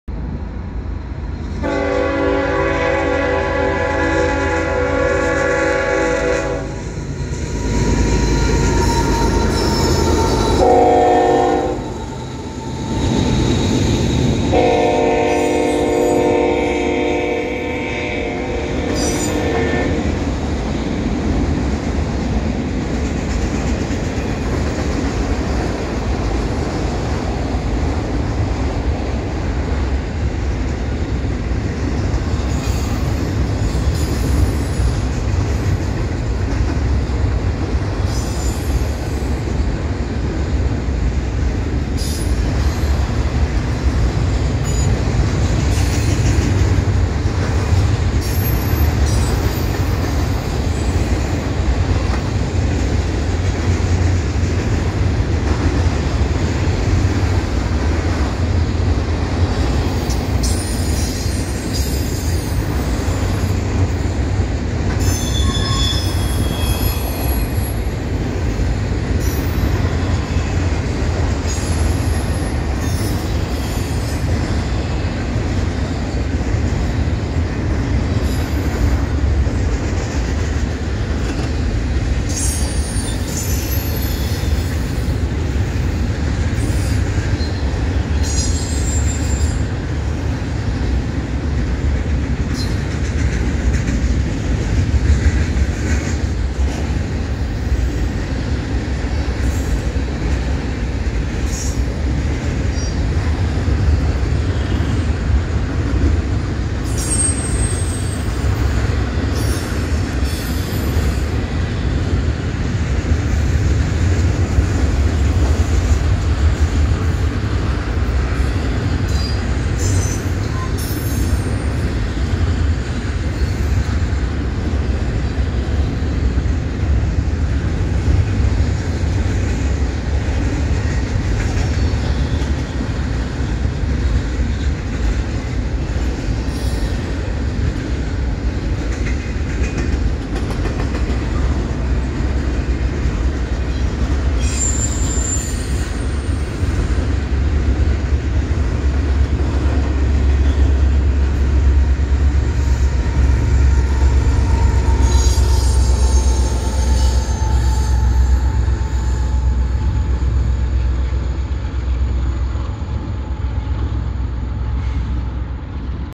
Union Pacific 7677 East hauling an autorack/intermodal with UP 2010 bringing up the rear.
Eastbound autorack/Intermodal at Cridge Ave in Riverside California.  Open door on UP 2010.